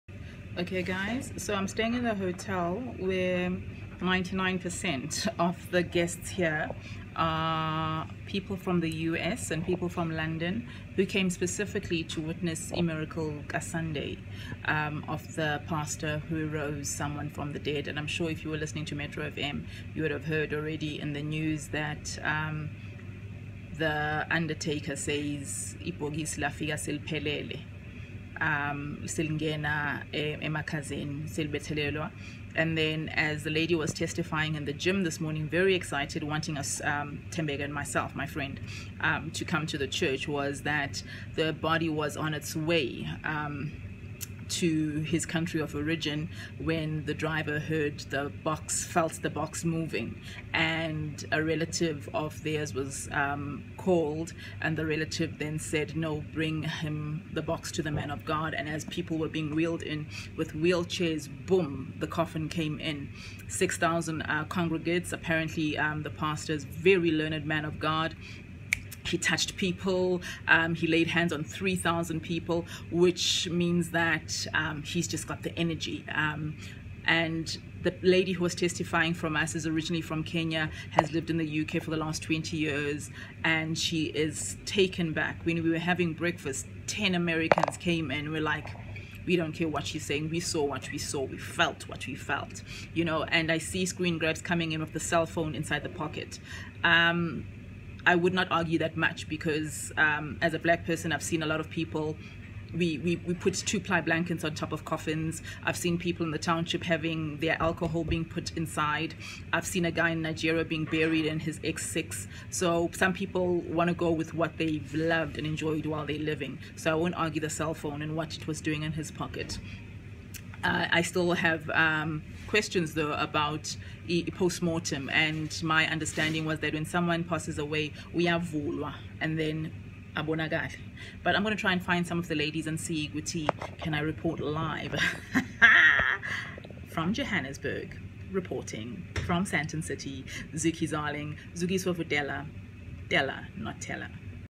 Life after Death
Zookey Zarling prepares to interview various delegates from Alleluia ministries